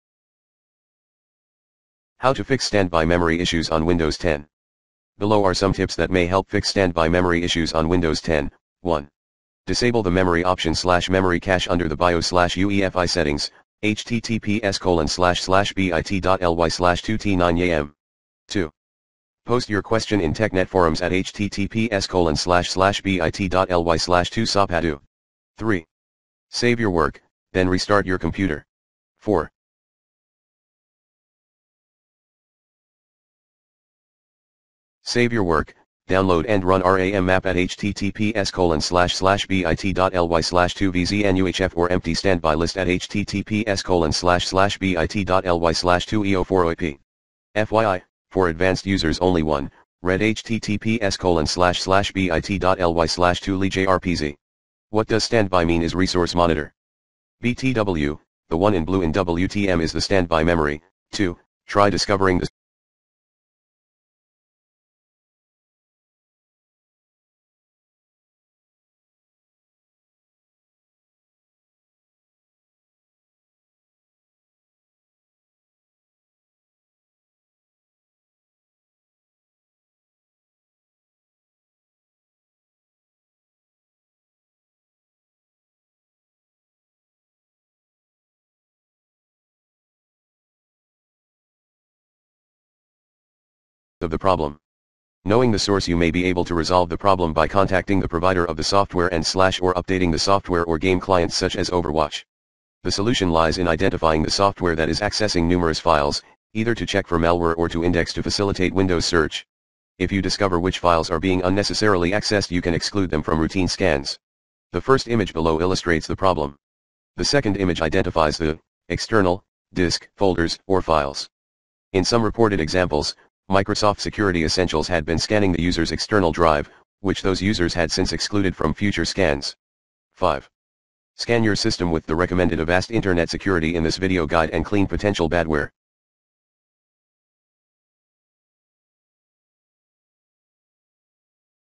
Fix standby memory issues on Windows 10
Here's how to Fix standby memory issues in your Windows 10. To effectively clear potential badware in your system, try Avast Internet Security@